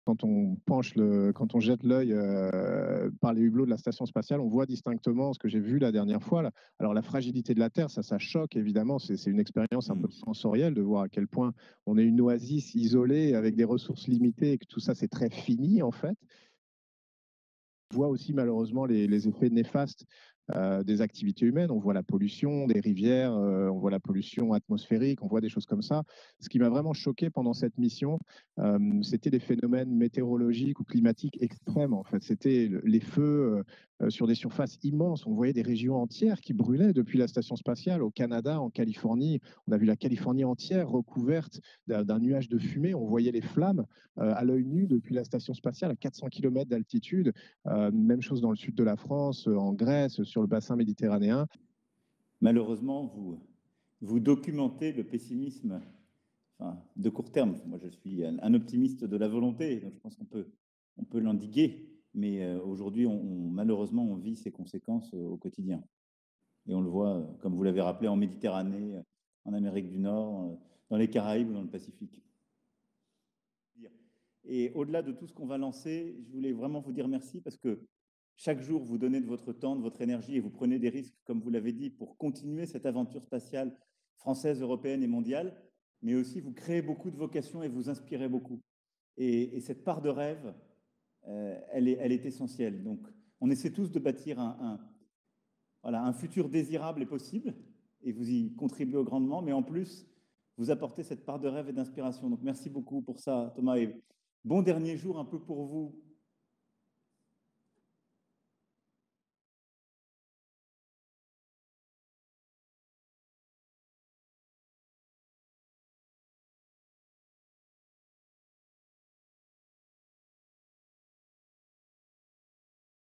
Thomas Pesquet décrit à Emmanuel Macron les catastrophes climatiques vues de l'espace
Avant de quitter la station spatiale internationale pour regagner la Terre, Thomas Pesquet a discuté avec Emmanuel Macron des récentes catastrophes climatiques observées depuis l'espace. Un échange en visioconférence alors que se tient actuellement la COP26 à Glasgow.